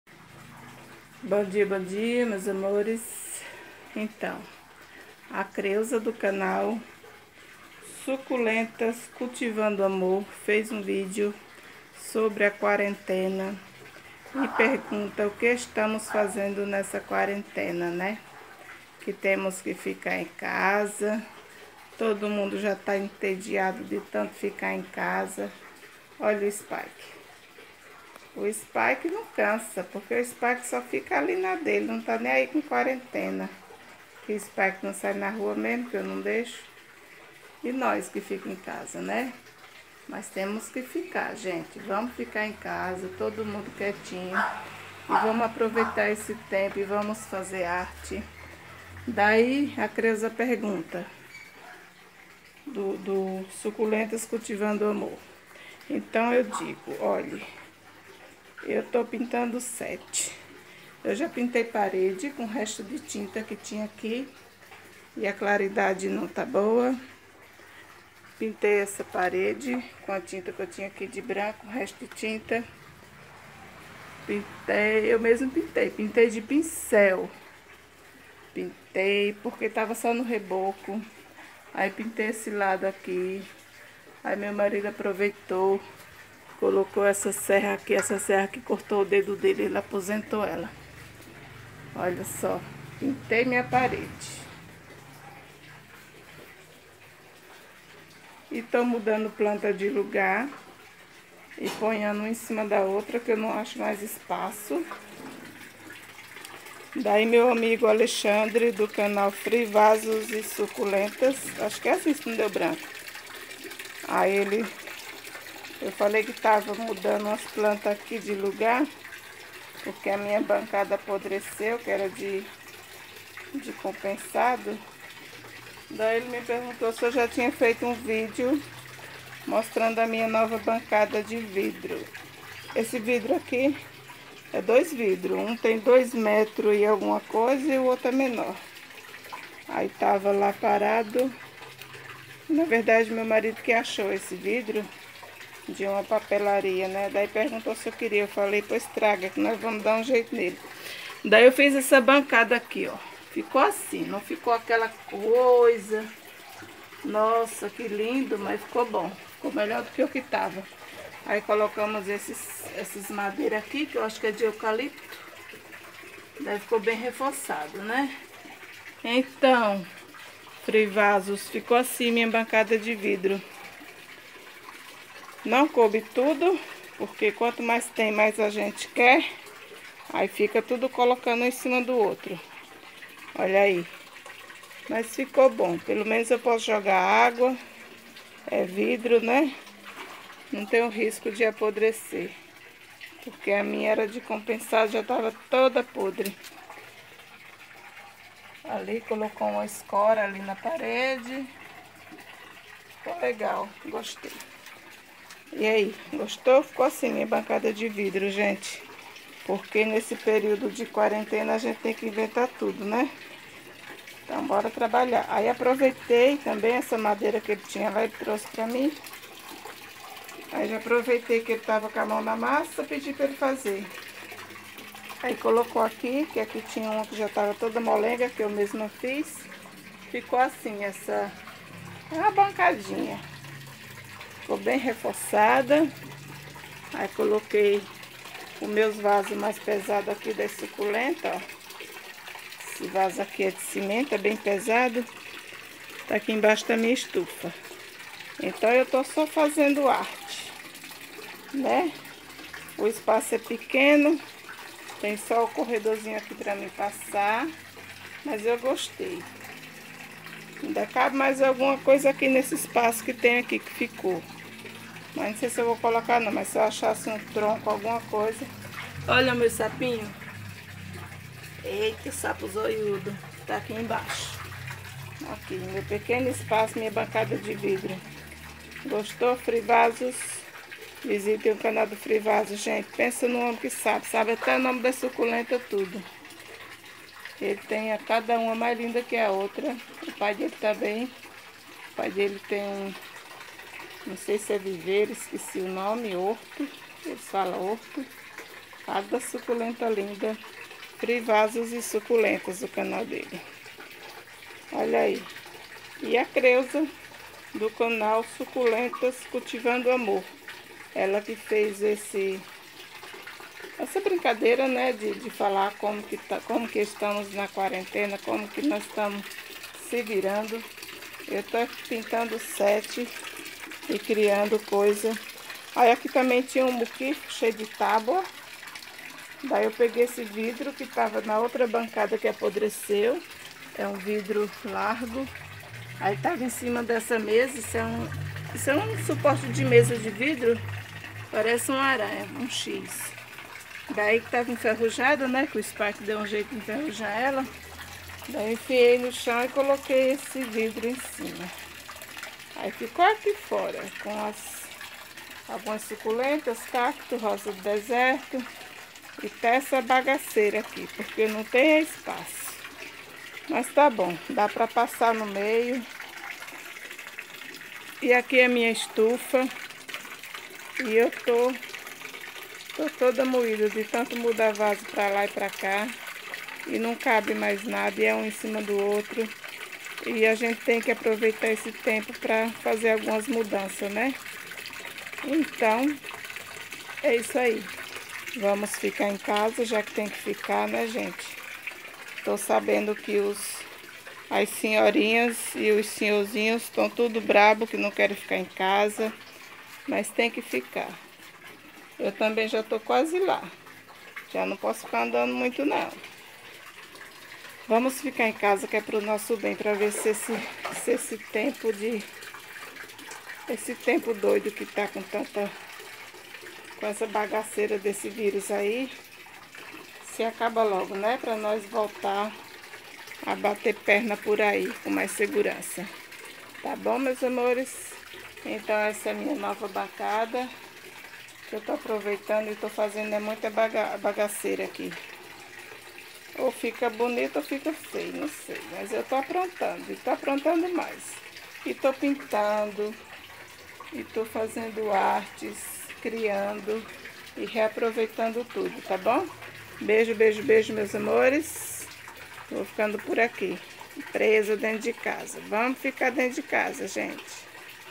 bancada de vidro, o que estou fazendo na quarentena?
Nesse vídeo eu mostro a Minha bancada de vidro, e o que estou fazendo pra ocupar o tempo na quarentena,